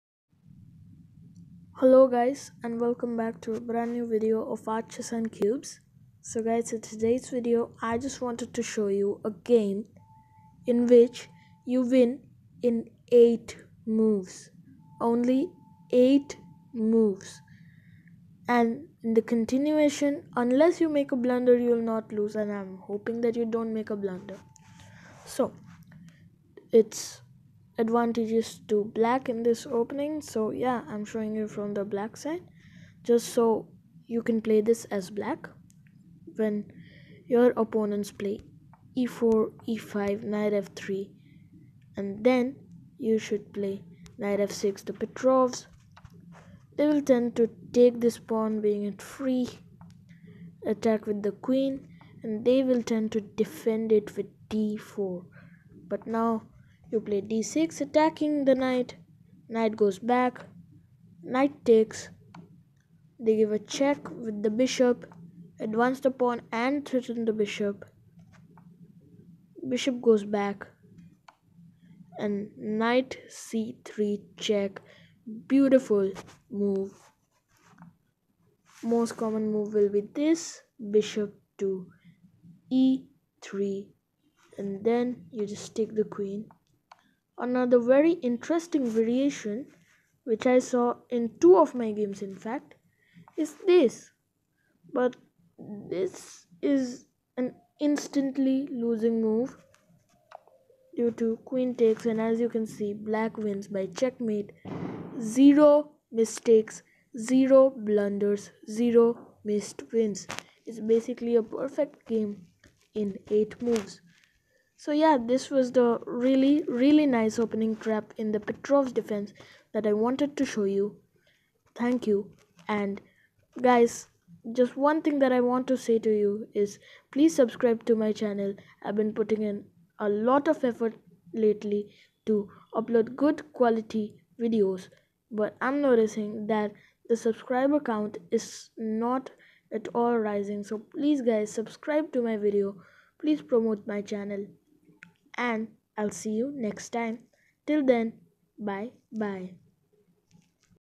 Chess Opening Traps Episode 5: The Petrov's Defence || Win The Game in 8 Moves
Video by Art, Chess and Cubes